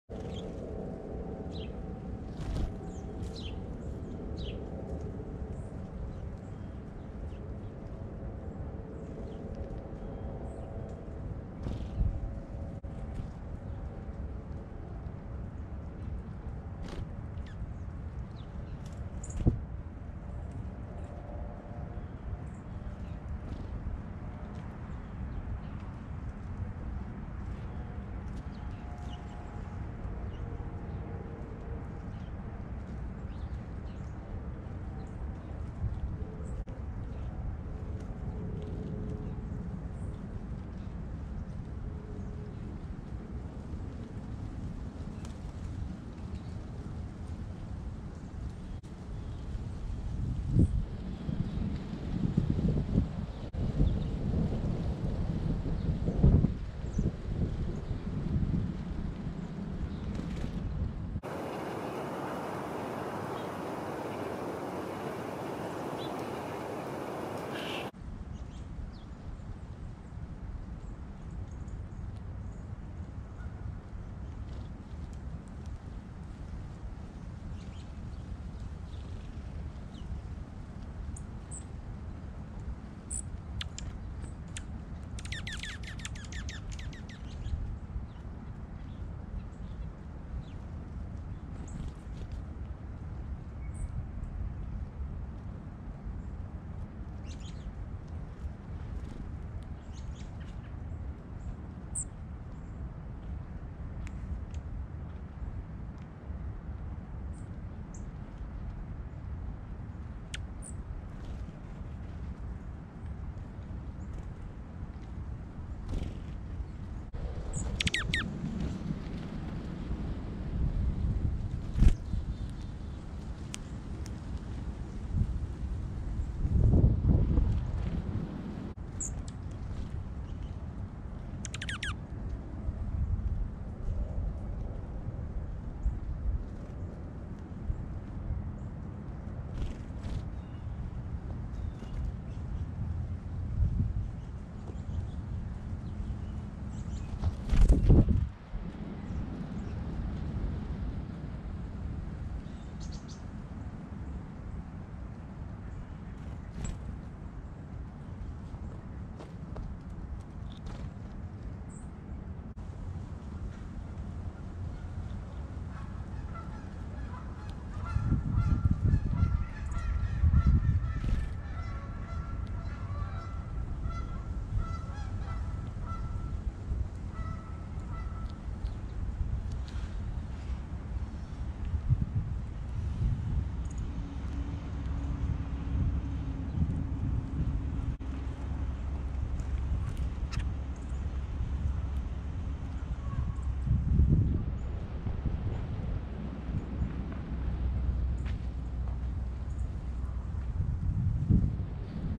I was happy to see a Tufted Titmouse back at Dufferin Islands with other feathered friends.
I spotted a Tufted Titmouse on my walk through Dufferin Islands feeding on a rock with Dark-eyed Juncos, Black-capped Chickadees, Blue Jays, Northern Cardinals and a Song Sparrow at the end of the video.
Jan 2023